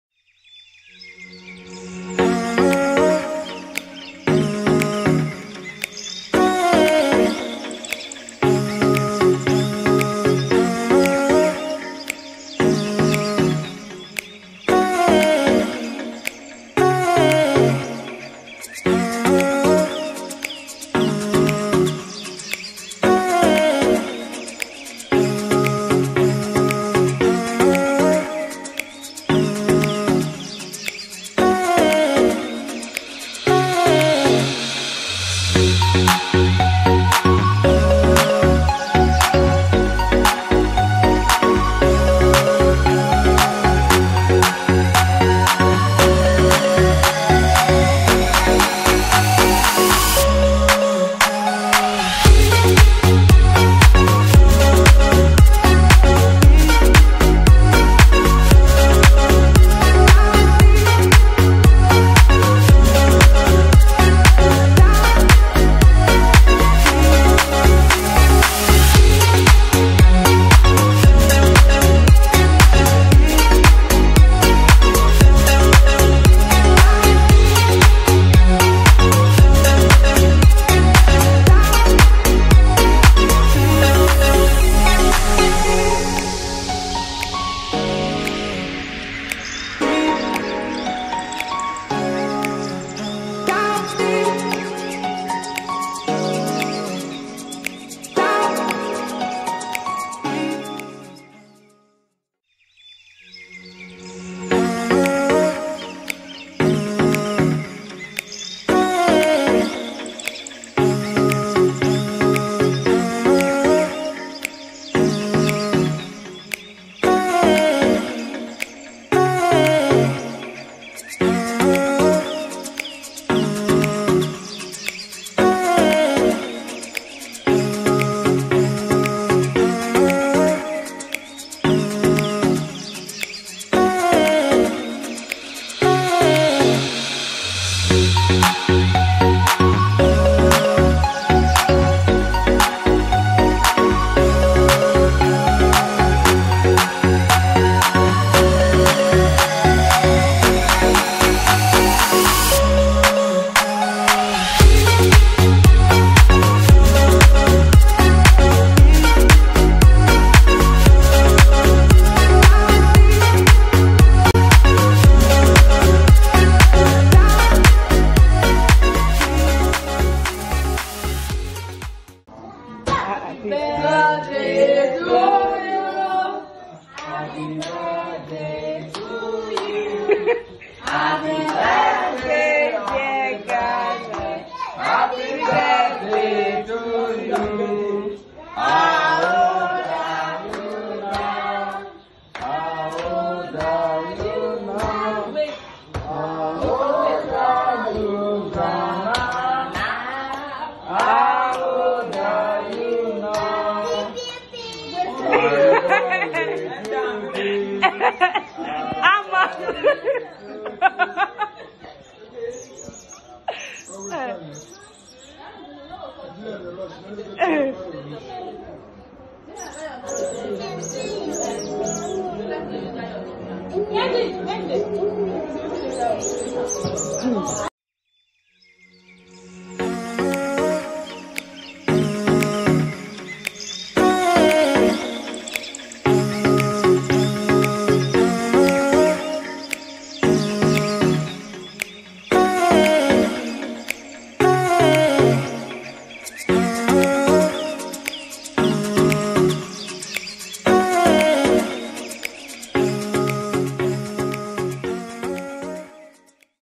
How we celebrated kanha's 4th birthday, kanha 4th birthday in Nigeria.
hi guys welcome to my channel, kanha became 4 years old on 4th of December,  we just called few family and friends that came to celebrate with him, it was a small but fun party.